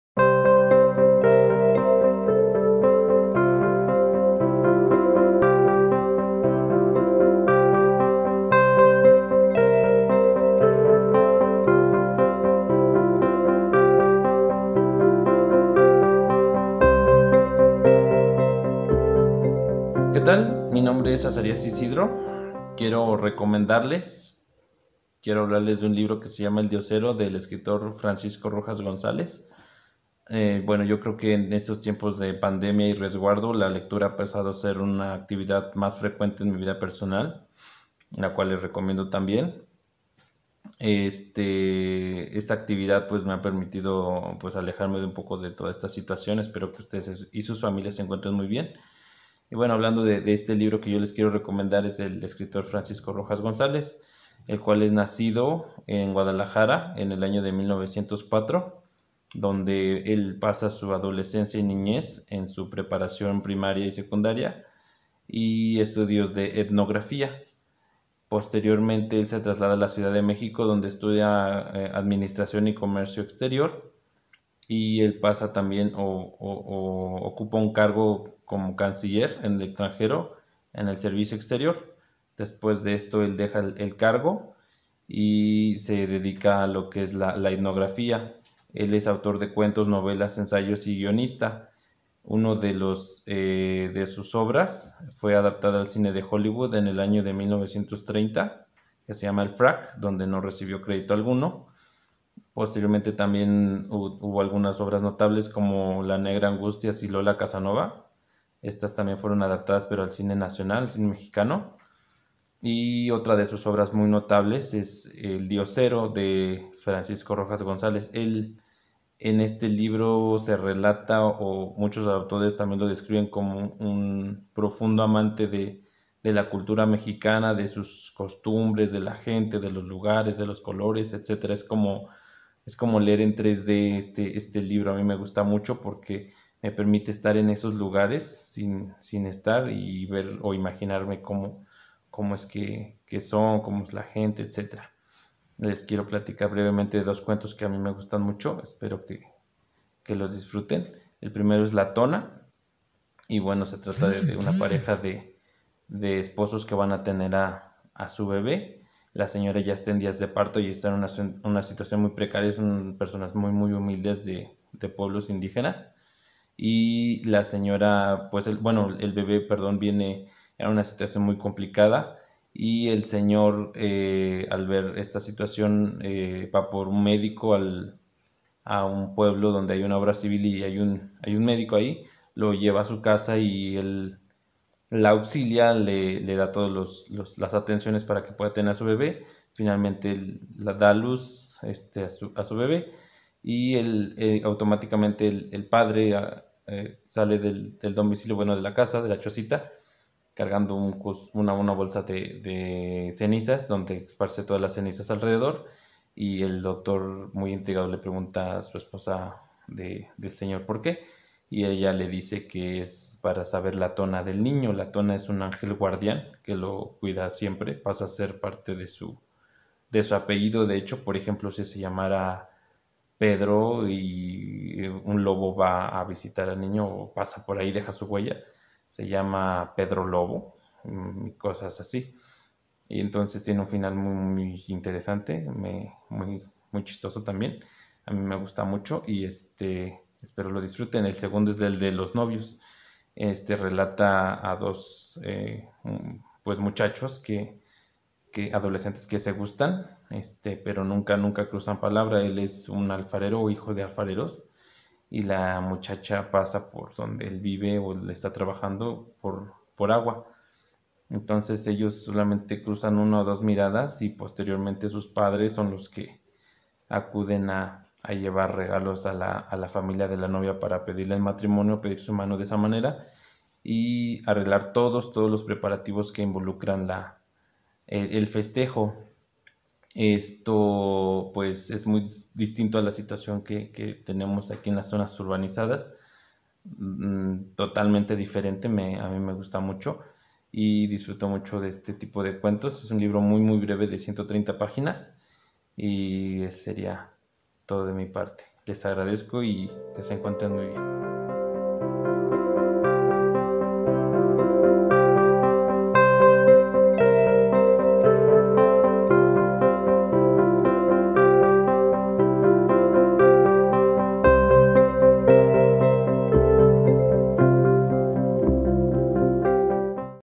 LEER es... dialogar en silencio. Buenas recomendaciones de buenas lecturas. (10)
🎥Te presentamos el décimo video de la serie: "LEER es... dialogar en silencio. Buenas recomendaciones de buenas lecturas."
Para disfrutar de lecturas recomendables. 🤓

📕El Diosero. Autor: Francisco Rojas González

🗣 Comentada por: Azarías Isidro. Estudiante de Ingeniería Mecánica, UAM-A